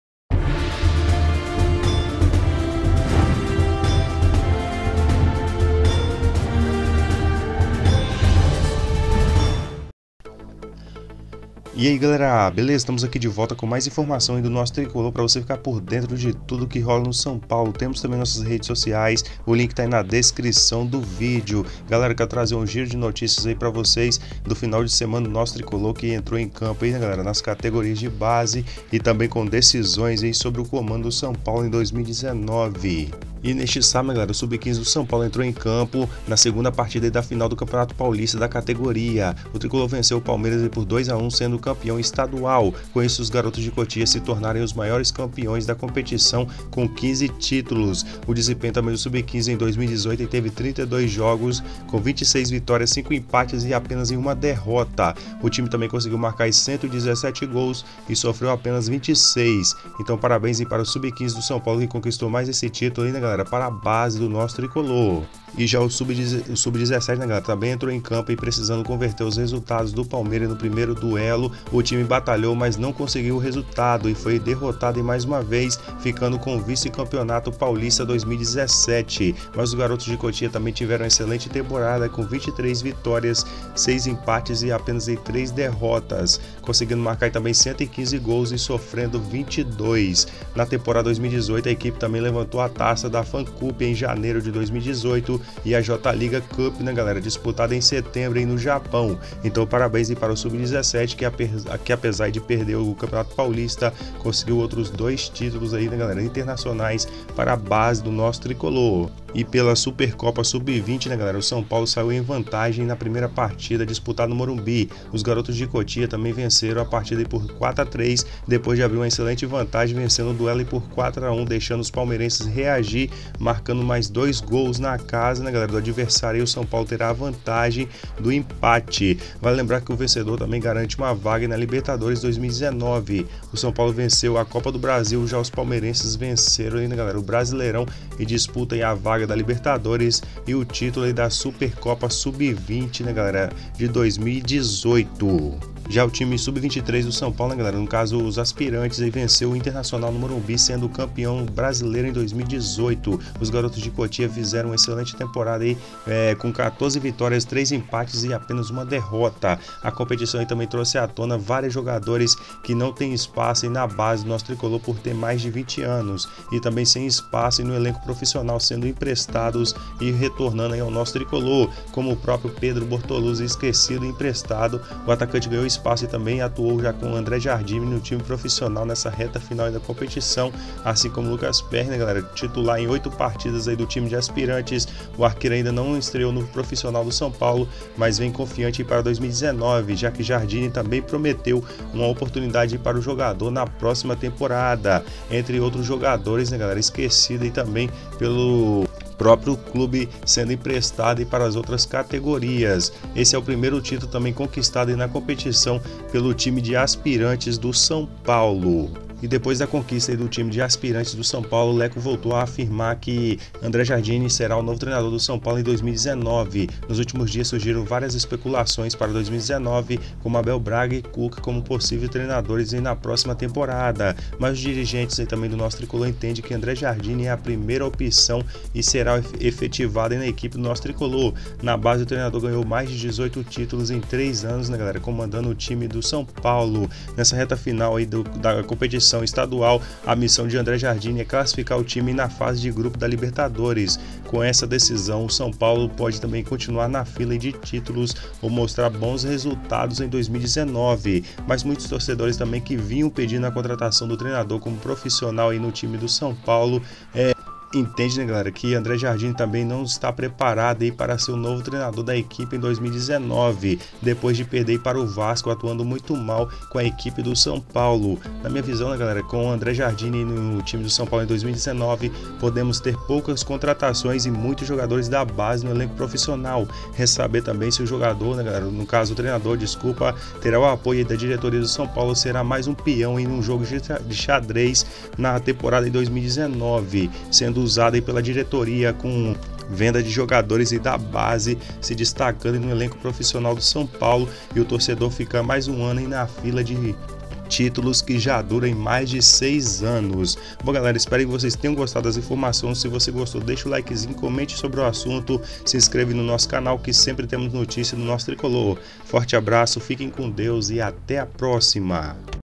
NOTICIAS ATUALIZADAS DO SÃO PAULO FC! O SABADO CAMPEÃO TRICOLOR, TREINADOR CONFIRMADO E MAIS
ACOMPANHE AS NOTICIAS  DO TRICOLOR NO CANAL

Fonte - São Paulo FC site